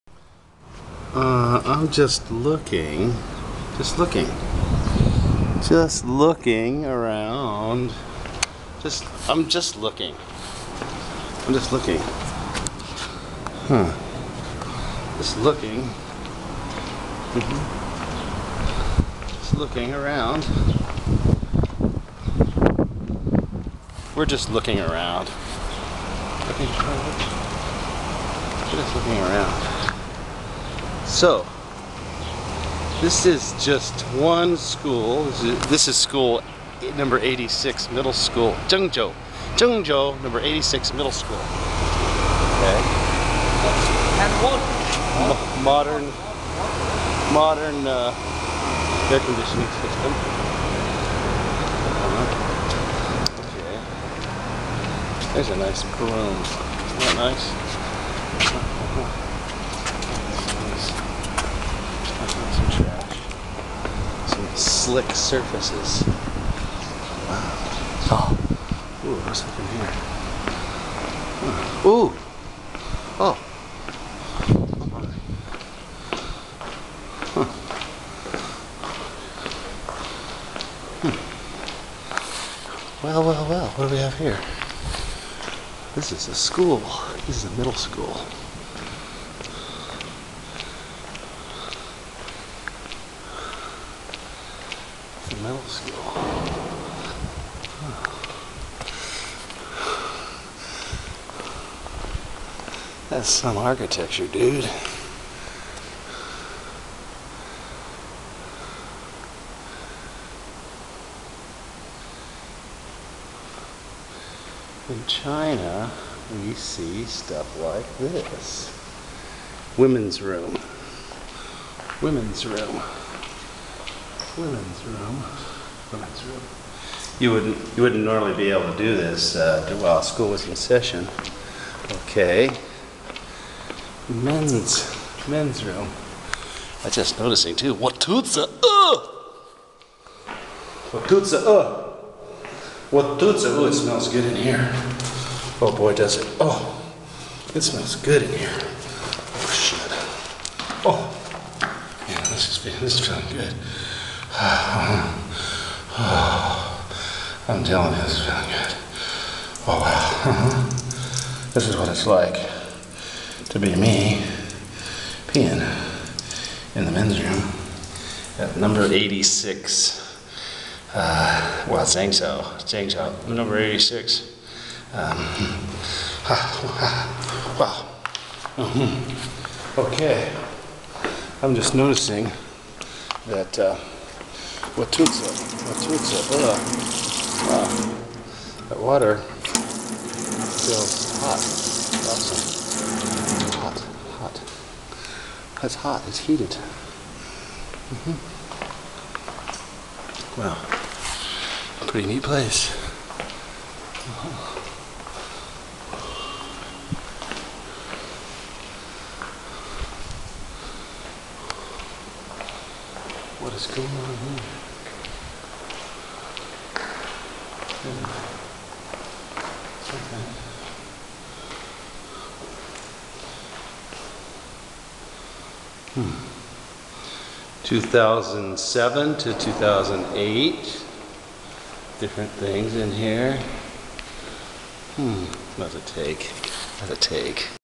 Touring Zheng Zhou No. 86 Middle School During Holiday
Place: Zheng Zhou No. 86 Middle School
Date taken: Wednesday, July 6, 2011 or the following day
File name: DSCN9953.AVI